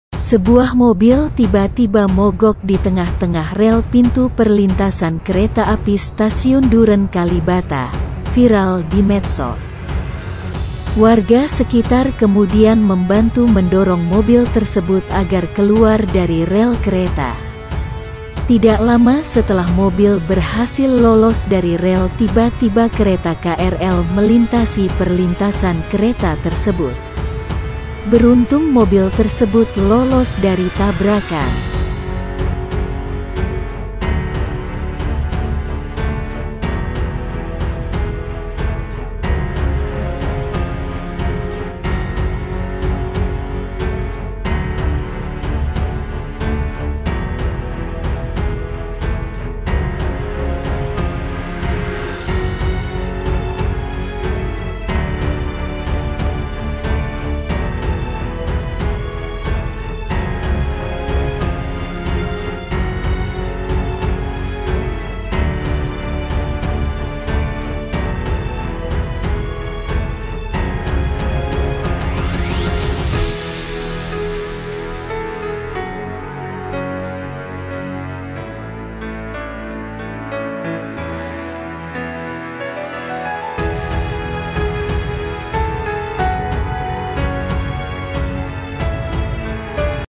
Viral Mobil Mogok di Tengah Rel Nyaris Tertabrak Kereta Api
TRIBUNLAMPUNG - Sebuah mobil tiba-tiba mogok di tengah tengah rel pintu perlintasan kereta api stasiun Duren Kalibata, viral di medsos.
Warga sekitar kemudian membantu mendorong mobil tersebut agar keluar dari rel kereta.
Tidak lama setelah mobil berhasil lolos dari rel tiba-tiba kereta KRL melintasi perlintasan kereta tersebut.
Beruntung mobil tersebut lolos dari tabrakan.